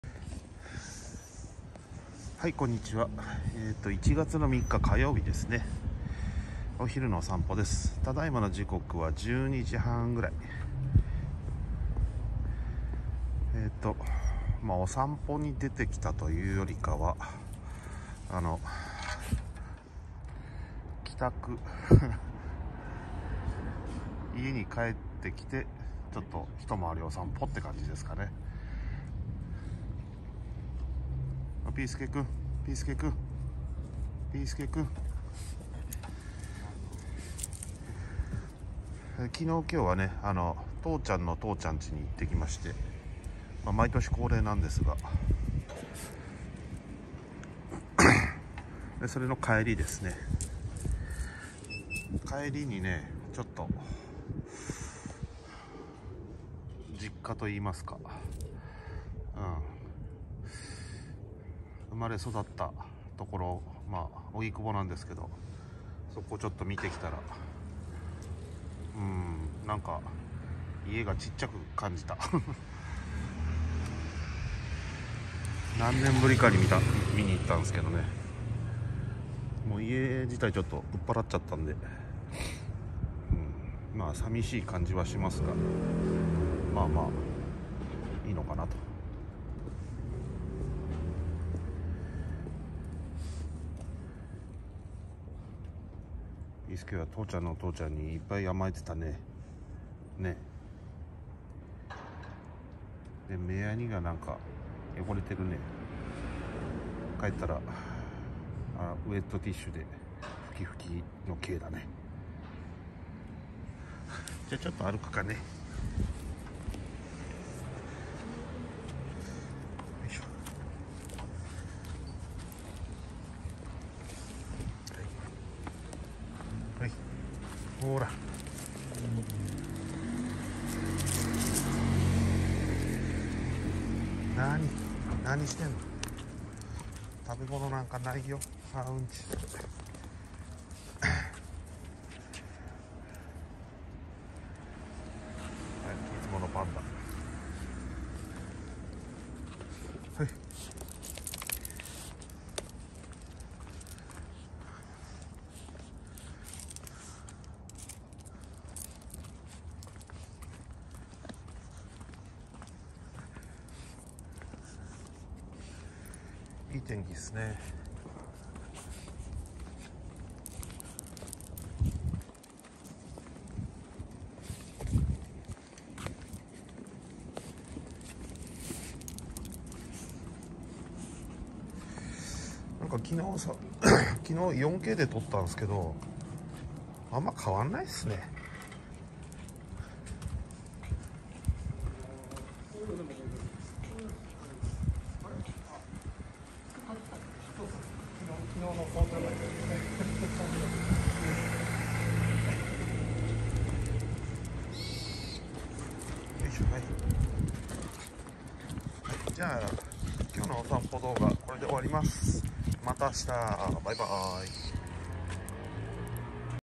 2023.01.03のお散歩217秒
17歳の吉宗くん（ミニチュアダックス）は2021年8月15日に永眠。
8歳のピョン吉くん（ミニチュアピンシャー）は2018年3月3日に永眠。
5歳のピースケくん（ミニチュアピンシャー）は元気印の三男坊。強面だけど甘えん坊だったりします。